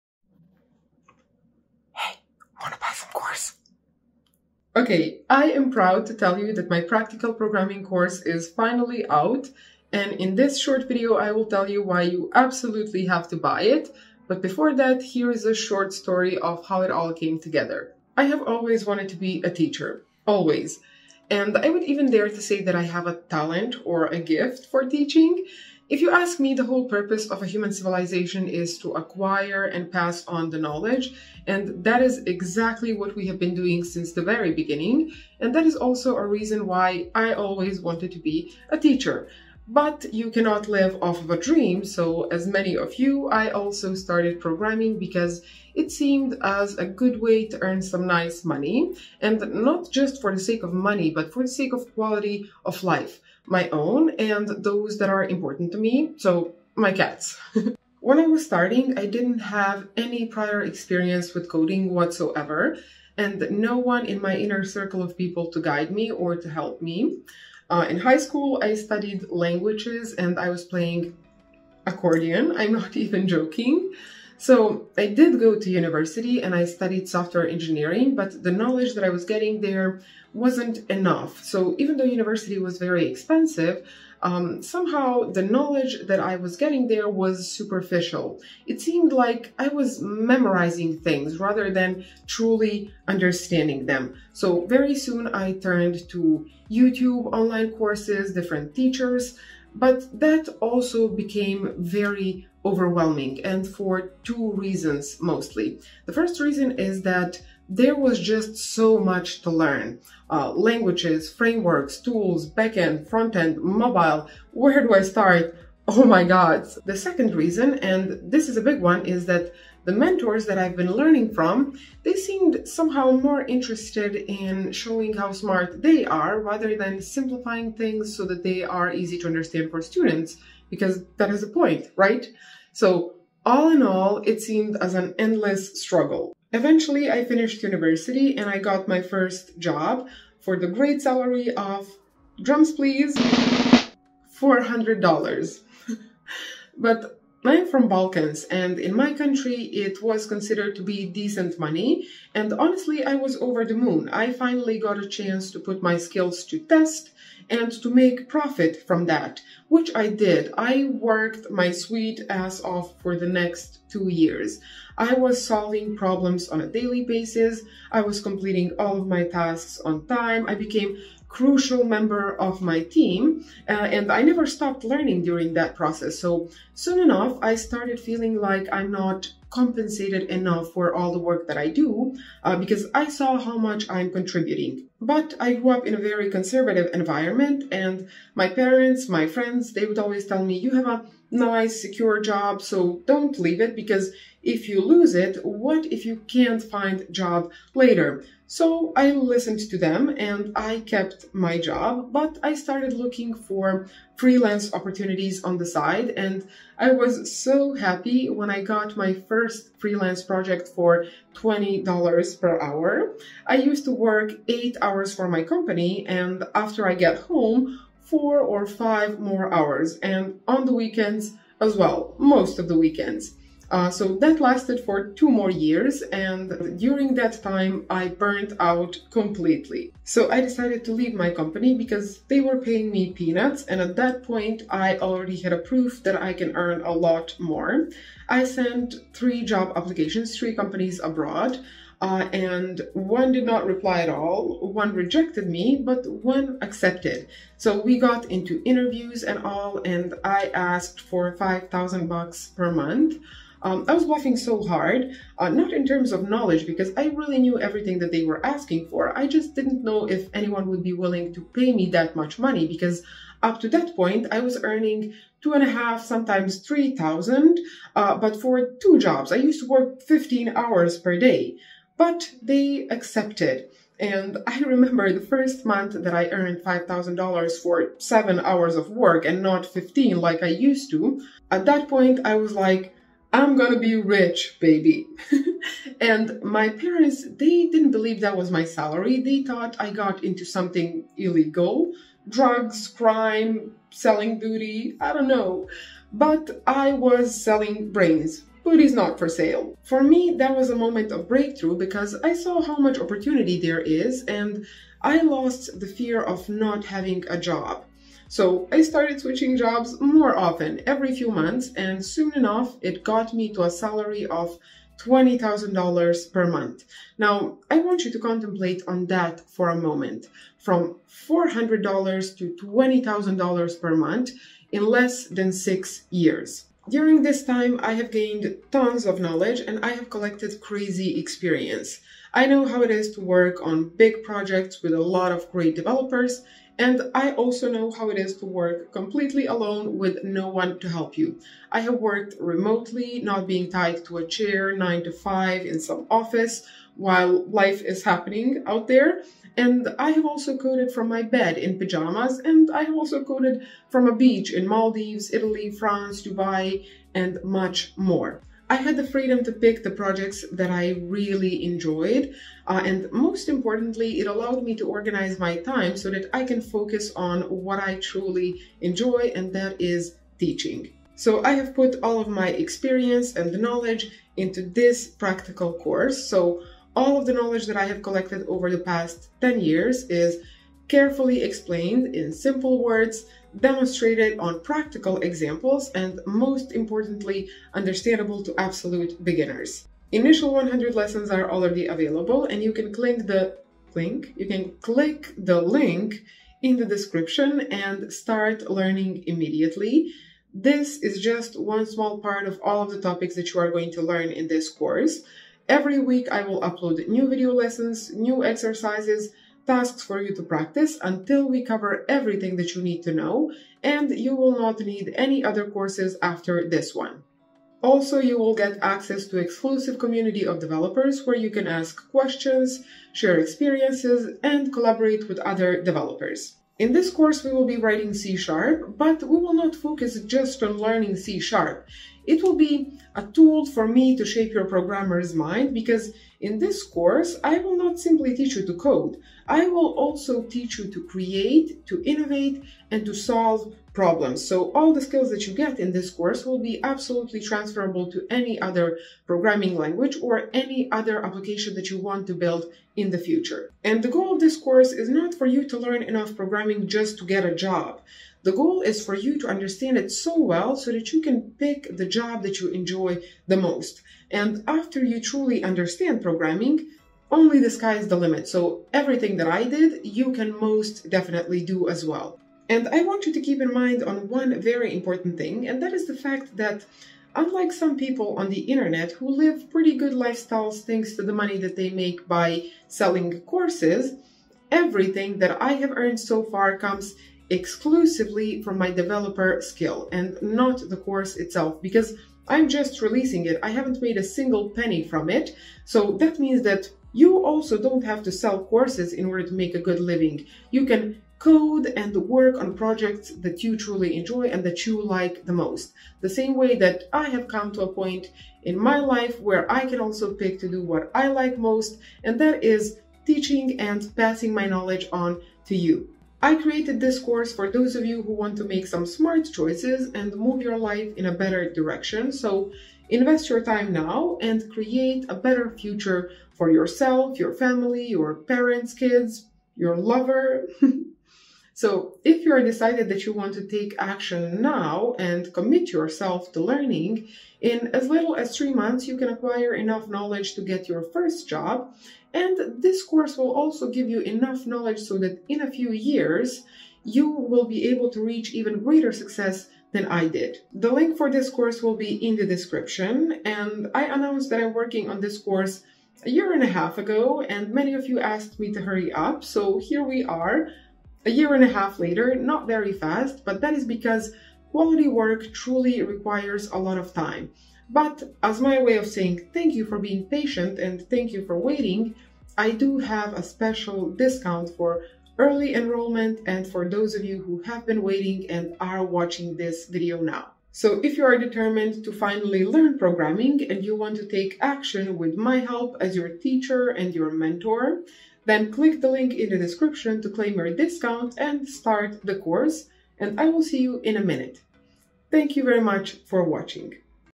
Learn to Code and Earn Money 💸 in Just 3 Months with this Beginner To Expert Programming Course
Experience the power of practical learning, gain career-ready skills, and start building real applications!
This is a step-by-step course designed to take you from beginner to expert in no time!
💰 Here is a coupon to save 10% on your first payment (CODEBEAUTY_YT10).
Use it quickly, because it will be available for a limited time.

This is a story of how I went from earning $400 to $20,000/month. In my Practical Programming course, I'll teach you how to transform your programming career and achieve the same or even greater success.

If your goal is to finish university - this course will give you comprehensive understanding of programming and help you to stand out among your peers!
Want to kickstart your career - you'll work on real-world projects and solve practical challenges to gain the experience you need in order to get a job that aligns with your passions, rather than settling for less!
You're creative and want to bring your app ideas to life - this course will give you all the necessary skills to do that!

Initial 100 lessons are already available and every week I'll upload new video lessons, exercises, and tasks for you to practice, until we cover everything that you need to know to start building real apps.

Plus, you'll get access to exclusive community of developers, so that you can ask questions, share experiences & collaborate with other developers.

(Make sure to apply your coupon on checkout to save your money.)